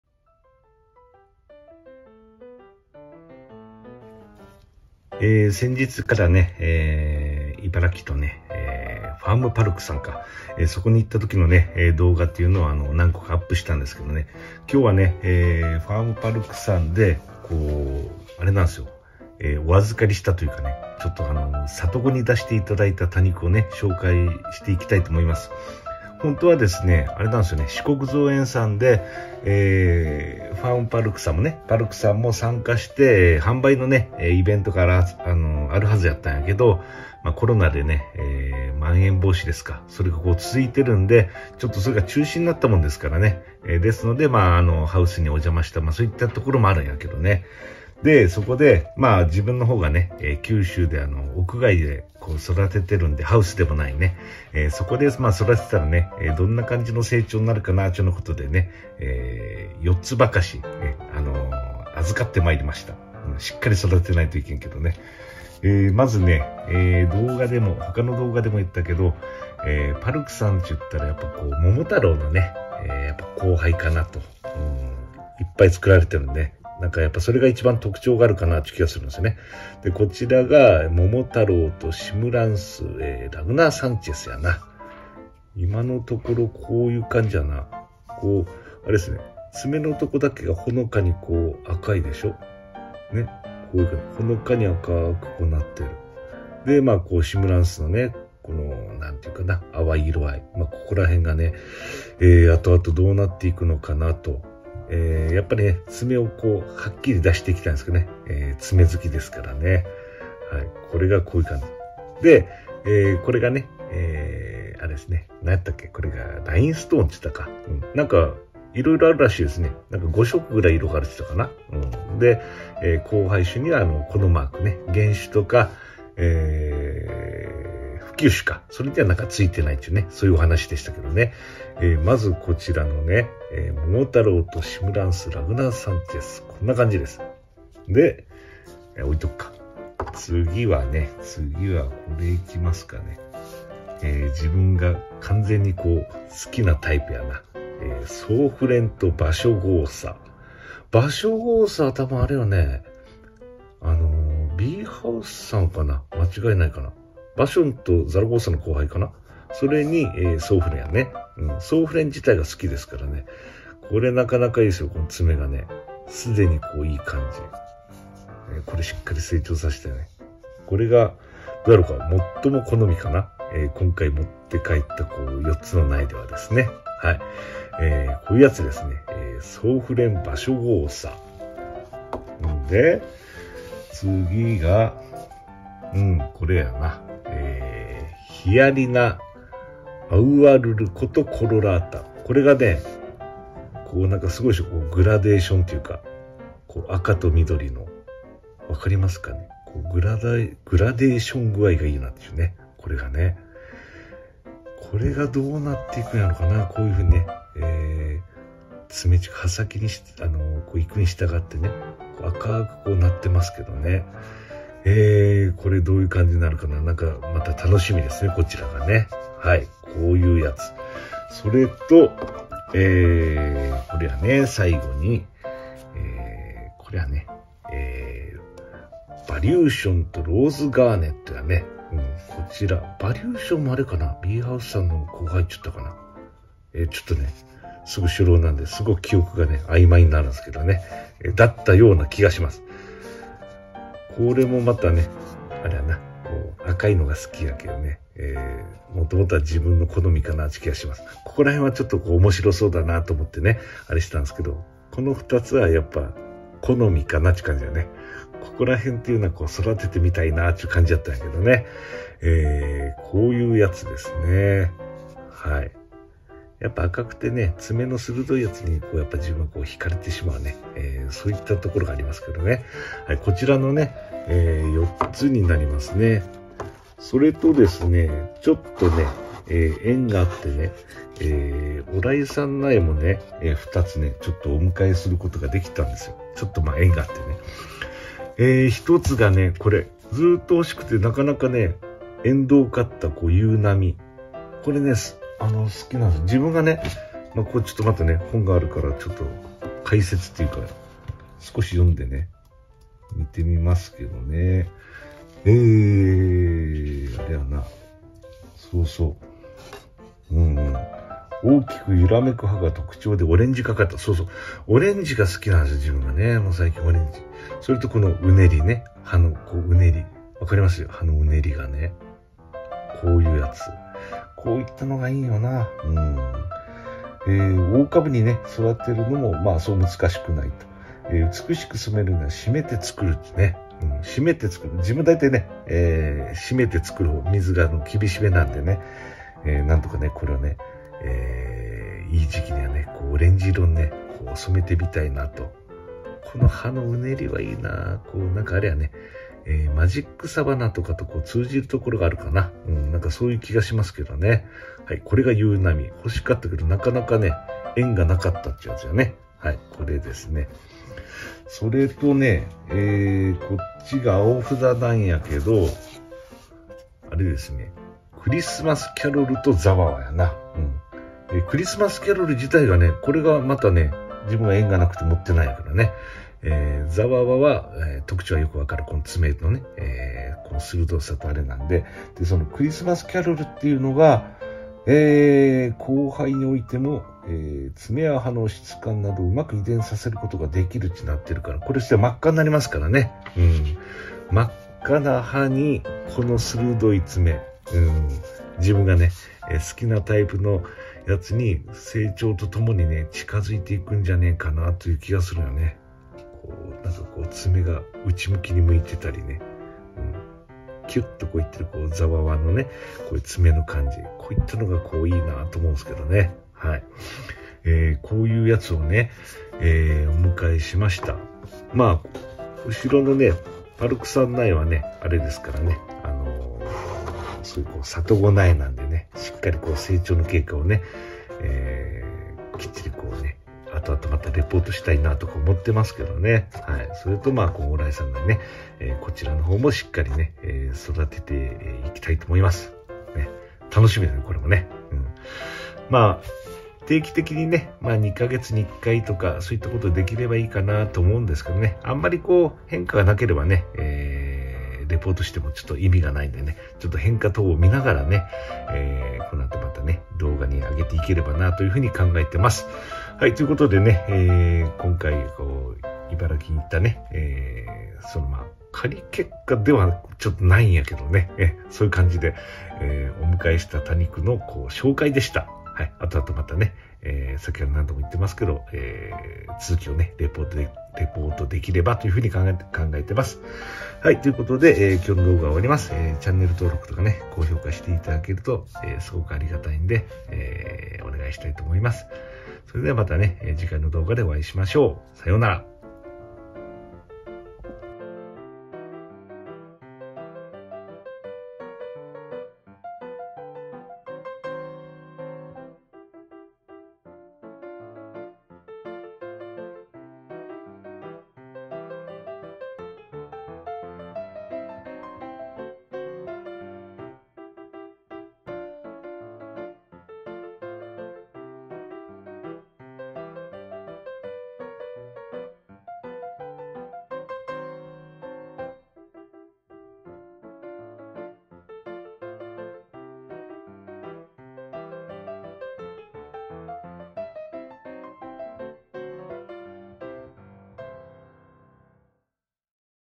【多肉植物】🌵里子をお預かりしました。😄 ＆おらいさん苗お迎え。
先日お伺いしたfarm.palkさんから
里子苗をお預かりしました。
環境のちがいによる
生長経過を今後レポートして
いければと考えています。😄

縁があって
お迎えしたおらいさん苗も
ご紹介しています。😄

【iPhoneによるメンバーシップ登録の方法は